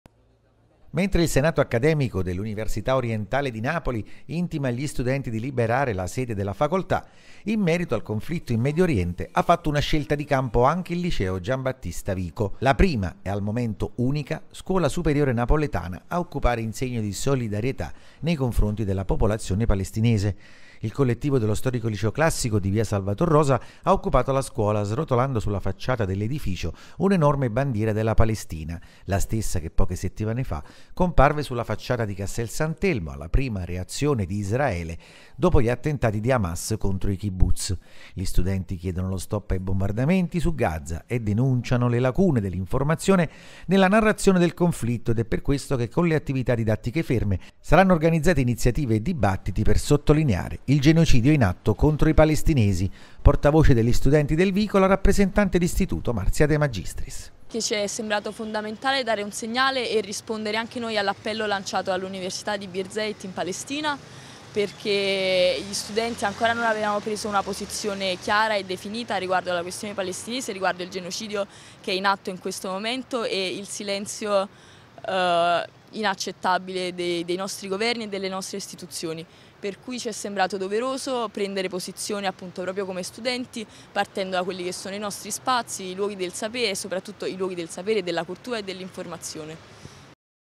Solidarietà alla Palestina, a Napoli occupato liceo G.B.Vico
Mentre il Senato Accademico dell’Università Orientale di Napoli intima agli studenti di liberare la sede della facoltà, in merito al conflitto in Medioriente ha fatto una scelta di campo anche il liceo Gianbattista Vico. La prima, al momento unica, scuola superiore a occupare in segno di solidarietà nei confronti della popolazione palestinese. Il collettivo dello storico liceo classico di via Salvator Rosa ha occupato la scuola, srotolando sulla facciata dell’edificio un’enorme bandiera della Palestina. La stessa che, poche settimane fa, comparve sulla facciata di Castel sant’elmo alla prima reazione di Israele dopo gli attentati di Hamas contro i Kibbutz. Gli studenti chiedono lo stop ai bombardamenti su Gaza e denunciano le lacune dell’informazione nella narrazione del conflitto ed è per questo che, con le attività didattiche ferme, saranno organizzate iniziative e dibattiti per sottolineare il genocidio in atto contro i palestinesi. Portavoce degli studenti del Vico, la rappresentante d’Istituto, Marzia de Magistris